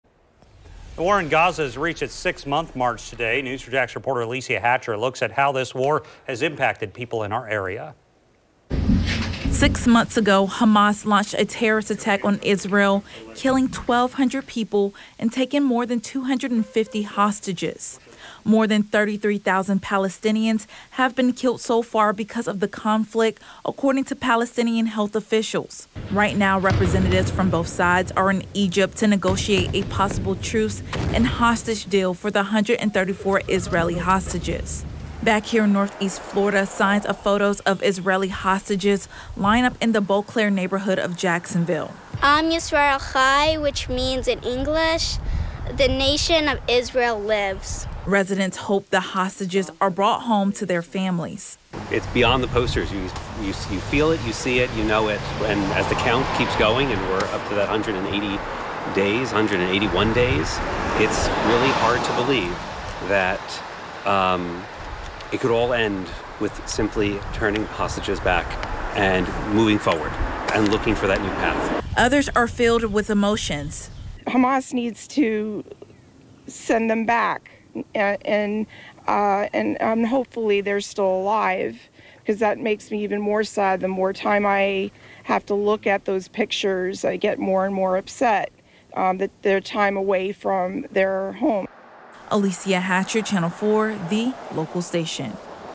Jacksonville residents express support for Israeli hostages in Gaza, posts photos around neighbo...
Six months into the war in Gaza, residents in a Beauclerc neighborhood voiced their support for Israeli hostages in Gaza by posting photos of them around the neighborhood.